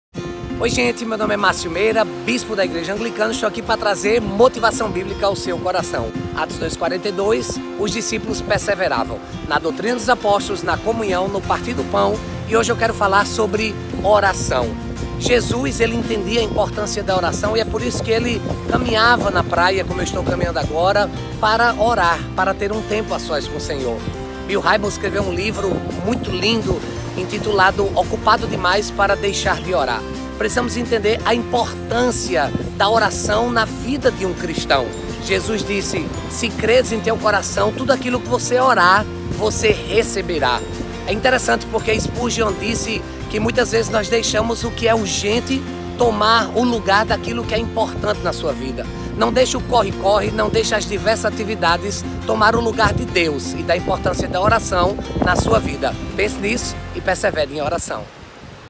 Persevere na oração
Série de vídeos devocionais do Bispo Márcio Meira da Igreja Anglicana com motivação bíblica para reflexão durante os 50 dias que antecedem o Pentecostes